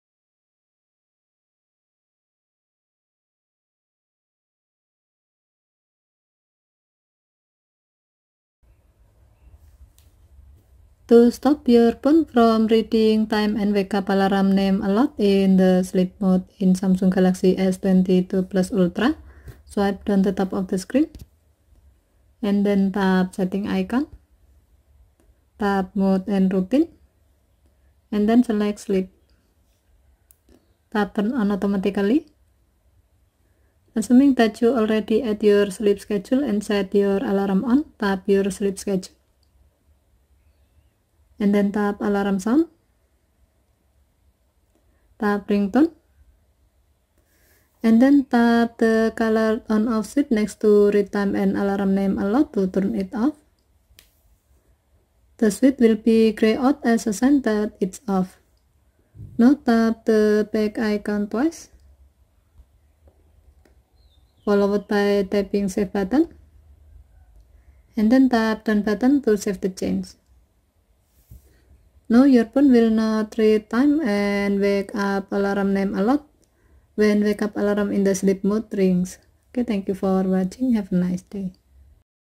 How to stop your phone from reading time and wake up alarm name aloud in the sleep mode
In this video, you will find the steps on How to stop your phone from reading time and wake up alarm name aloud in the sleep mode in Samsung Galaxy S22/S22+/S22 Ultra.  This video uses Samsung Galaxy S22 Ultra SM-S908E/DS International version runs with Android 12